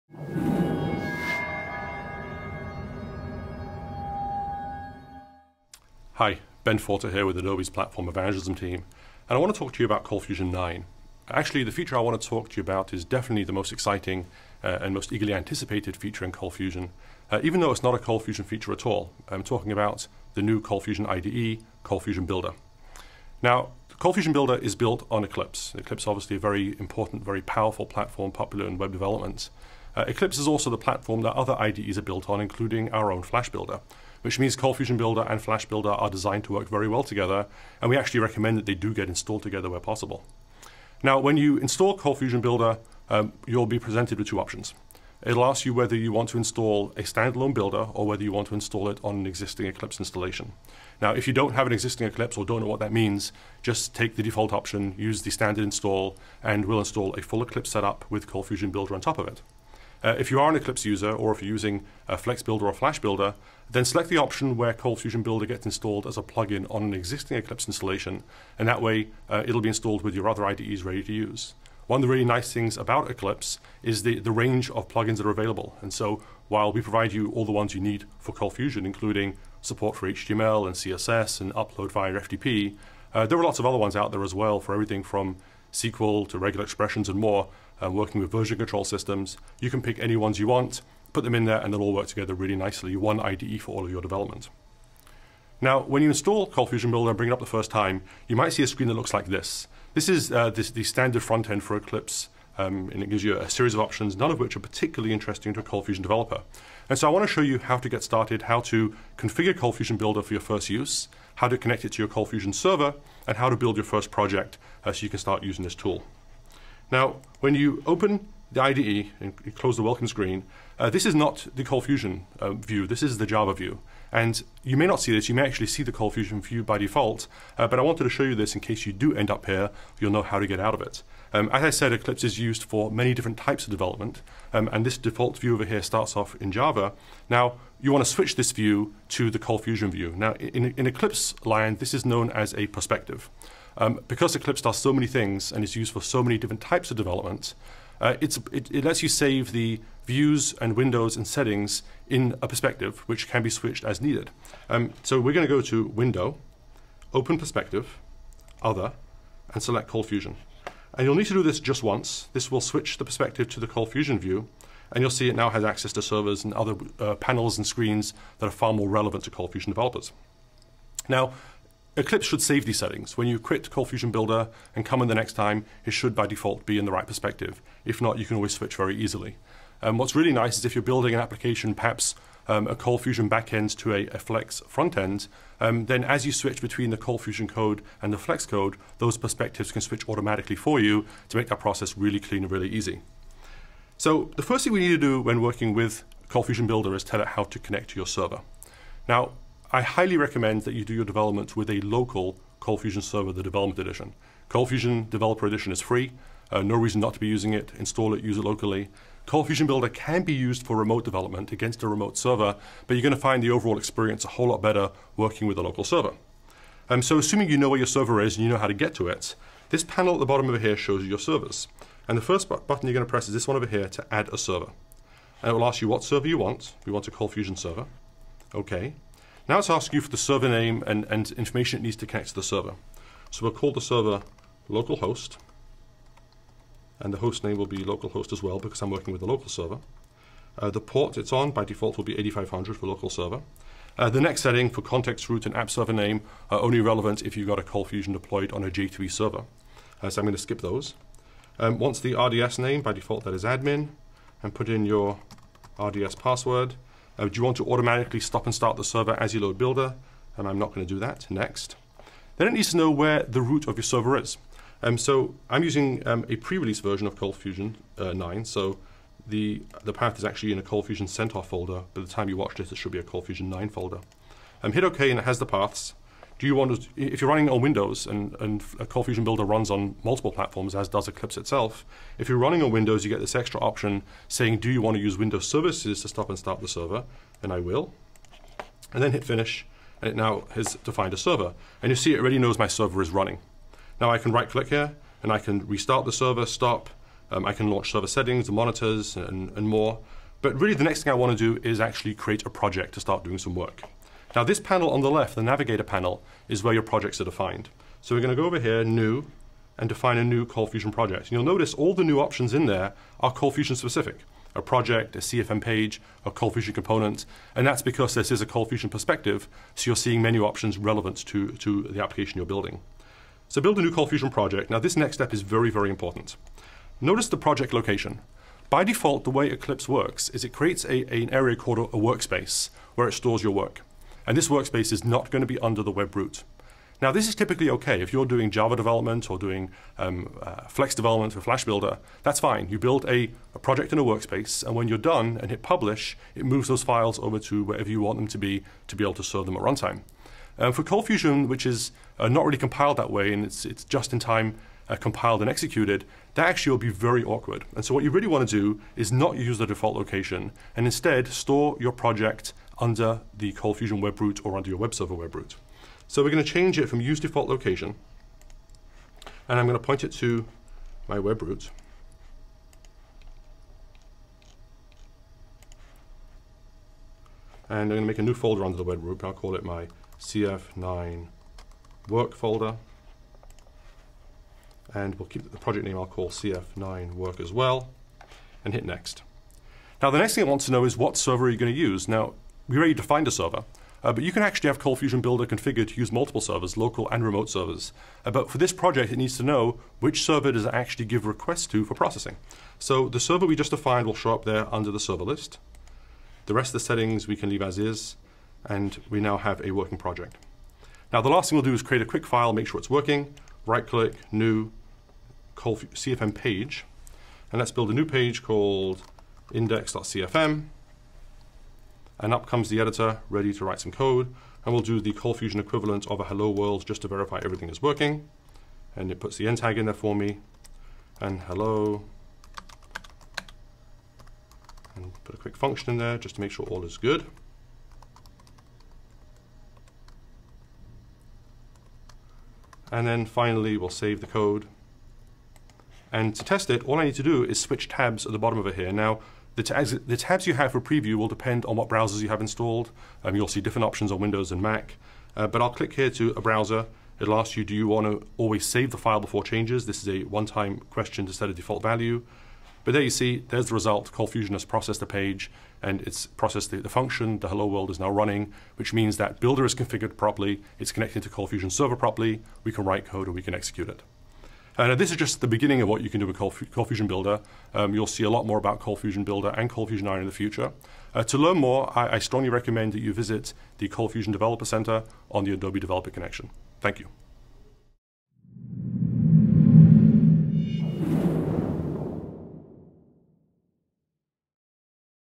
ADC Presents - Get Started with ColdFusion Builder
Learn about ColdFusion 9 and its new Eclipse-based IDE, ColdFusion Builder.  Adobe's Ben Forta explains how to create and configure your first ColdFusion Builder project.